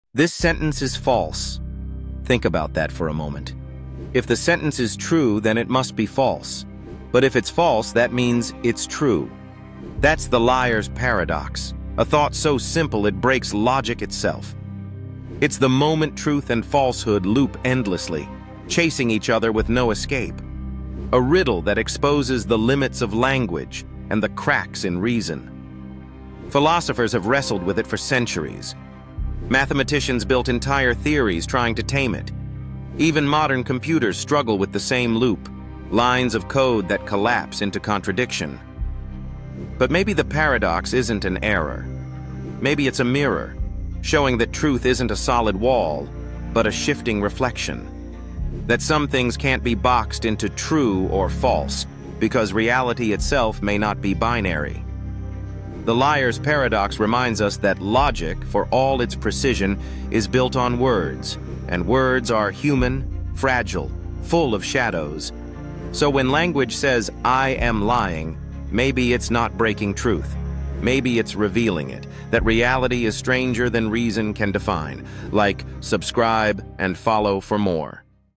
What Is the Liar’s Paradox?
“This sentence is false.” If it’s true, it’s false. If it’s false, it’s true. ♾️